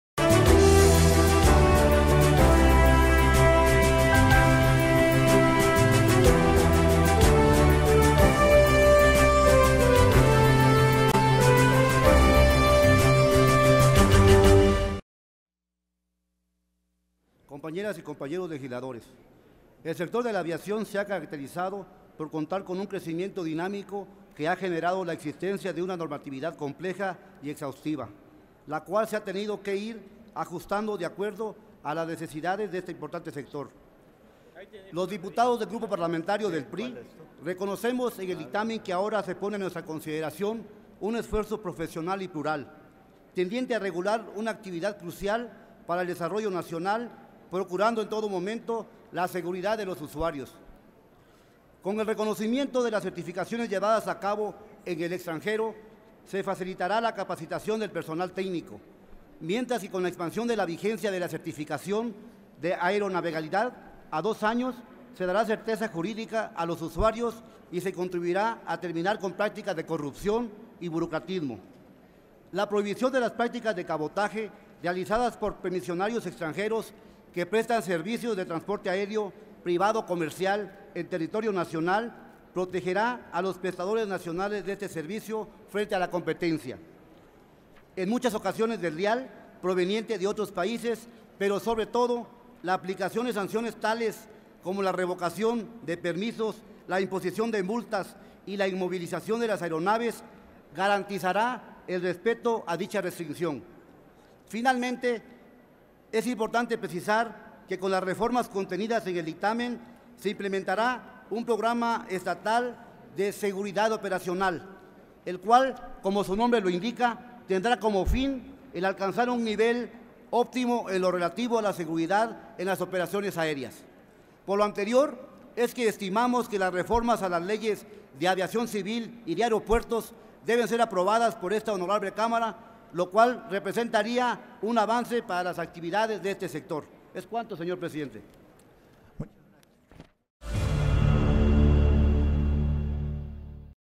Dip. Hugo Pérez (PRI) - Ley de Aviación Civil y Ley de Aeropuertos
El Dip. Hugo Mauricio Pérez Anzueto (PRI) señala que las reformas planteadas protegerán a los prestadores nacionales de estos servicios, y permitirán alcanzar un nivel óptimo en la operatividad de la industria aeronáutica del país.